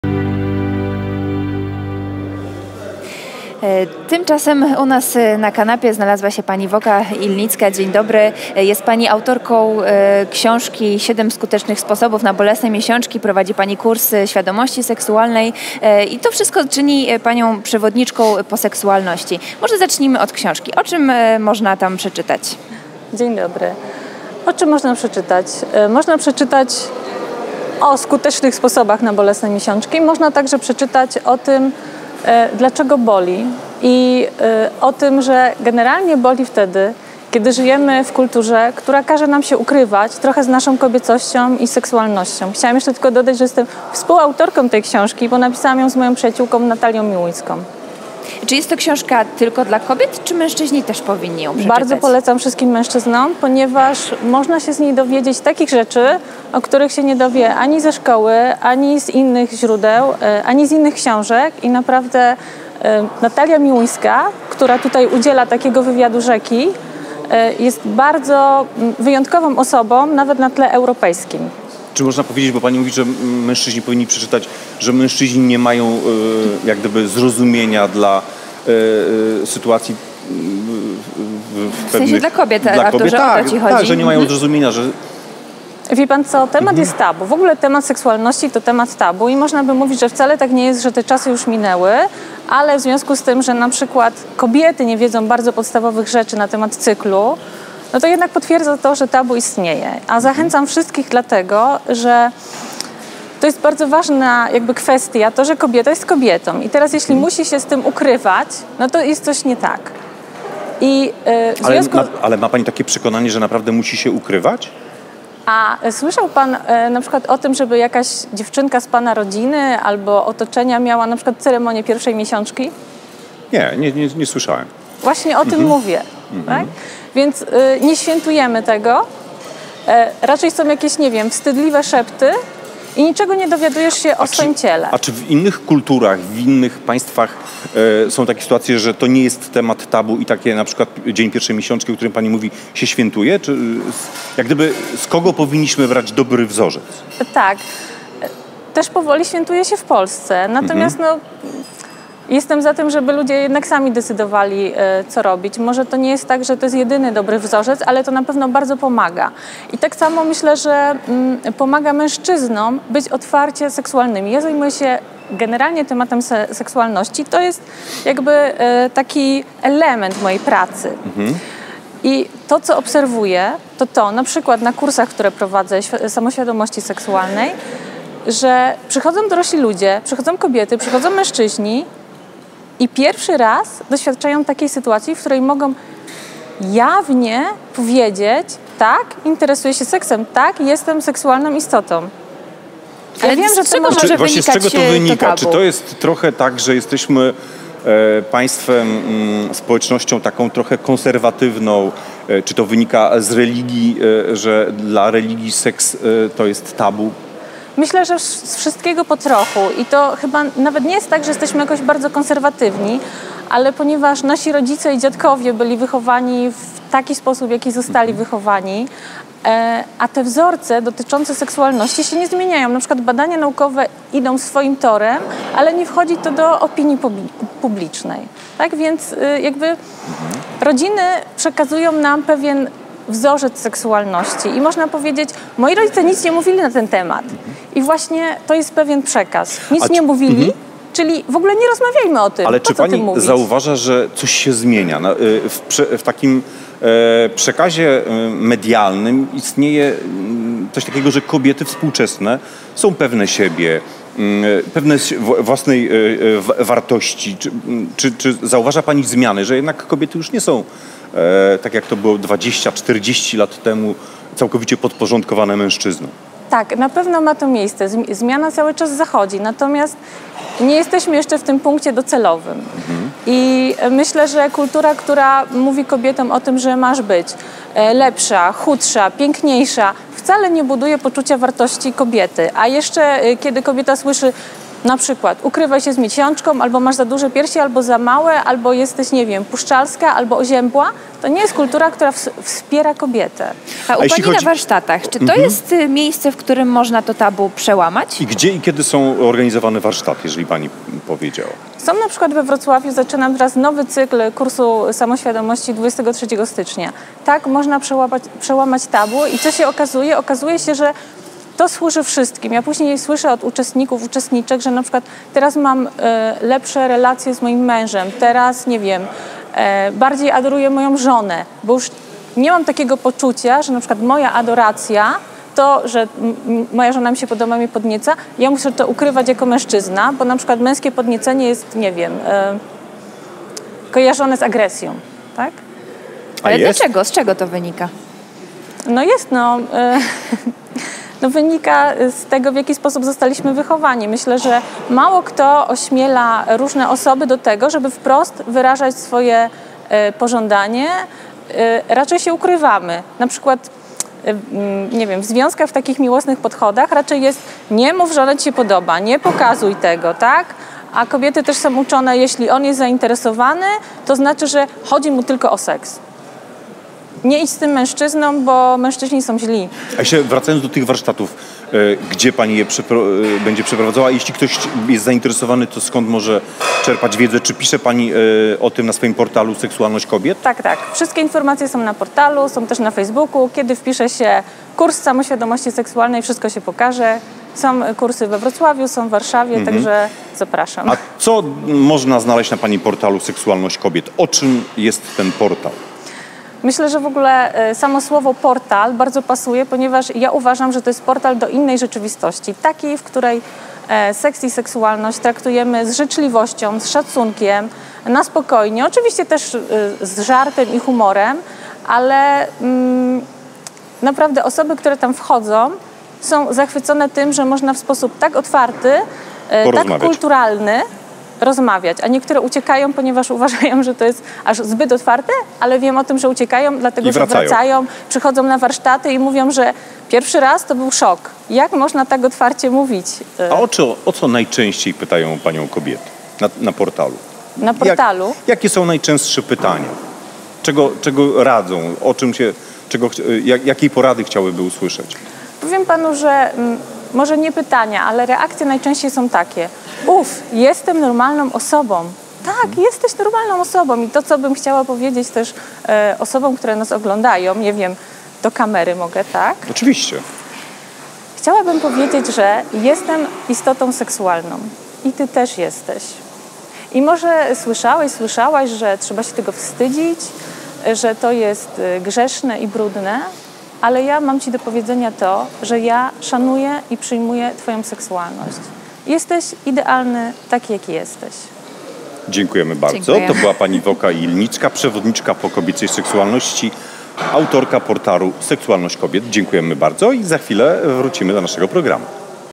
Kobieca seksualność w Echo24 TV
Voca Ilnicka opowiada o portalu, warsztatach, książce i apeluje do Istot Seksualnych